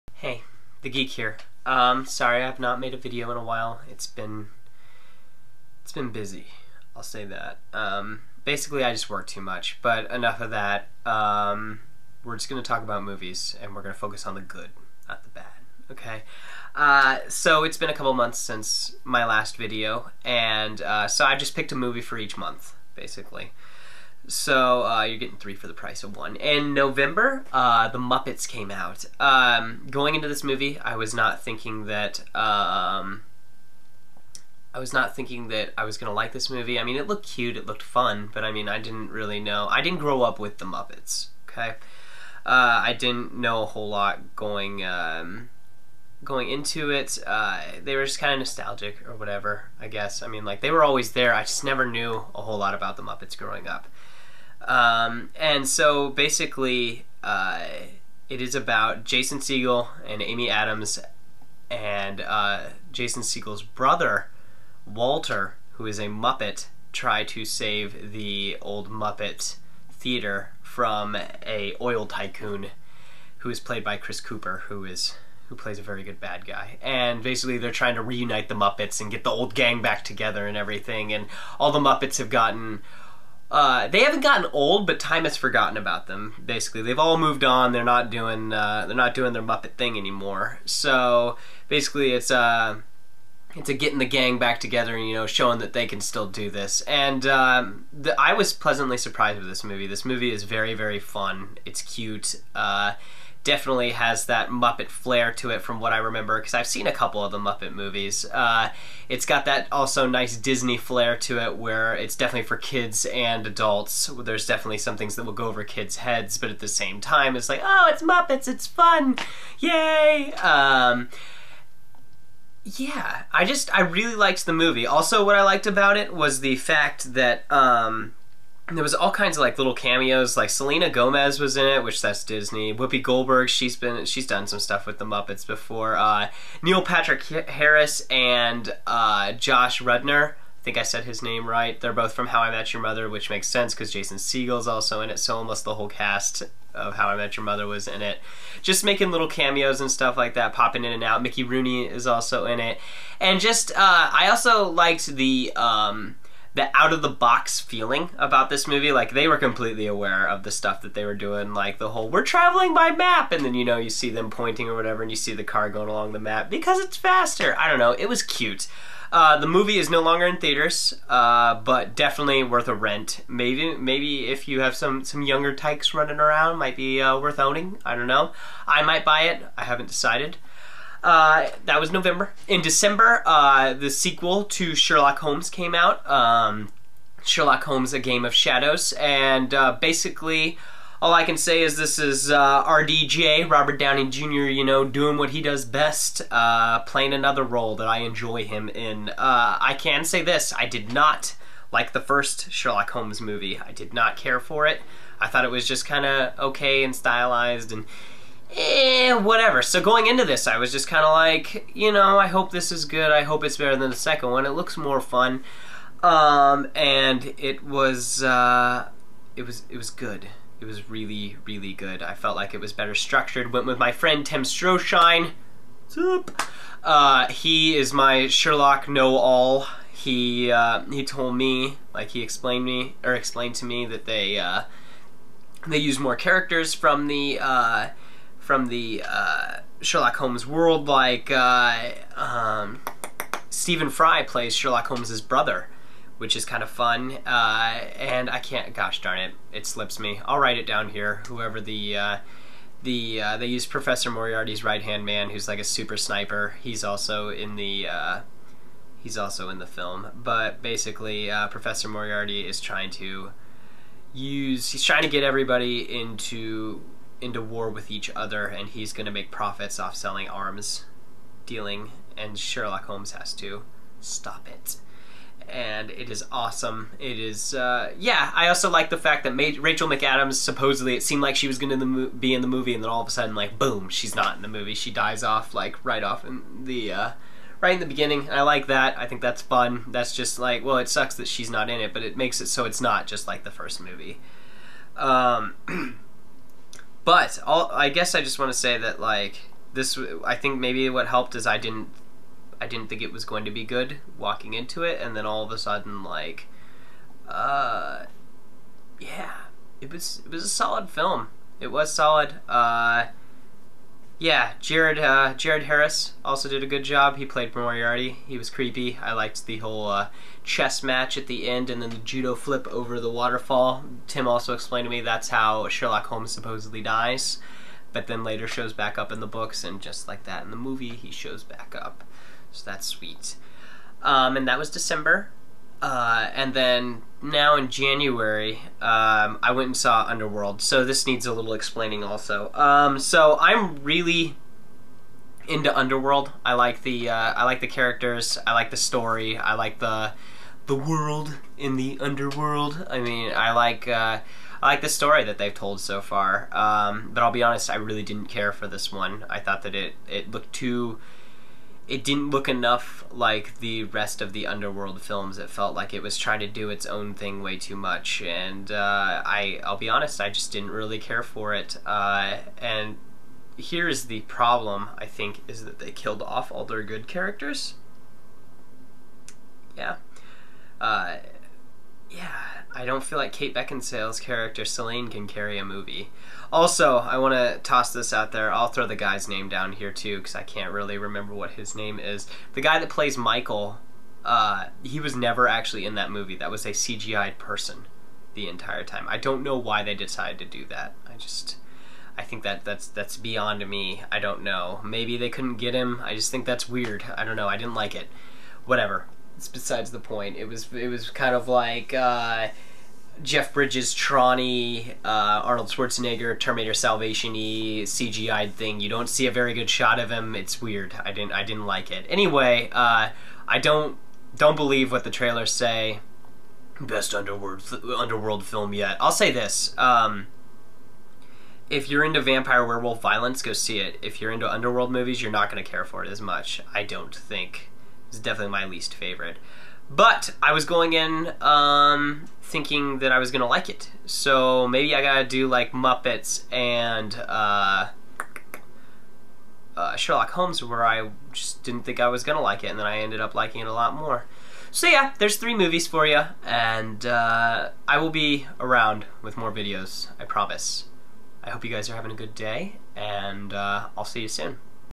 Geek Nation Episode IV: The Muppets, Sherlock Holmes, and Underworld thoughts
Thoughts on the films and yes a few spoilers.